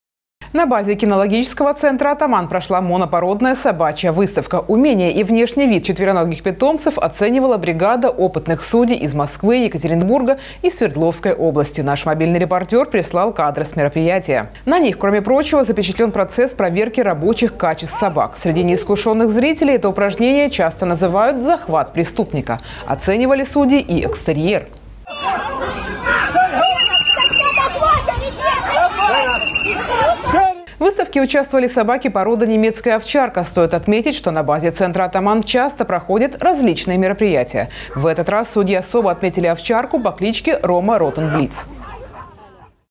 МОБИЛЬНЫЙ РЕПОРТЁР
На базе кинологического центра «Атаман» прошла монопородная собачья выставка. Умения и внешний вид четвероногих питомцев оценивала бригада опытных судей из Москвы, Екатеринбурга и Свердловской области. Наш мобильный репортёр прислал кадры с мероприятия.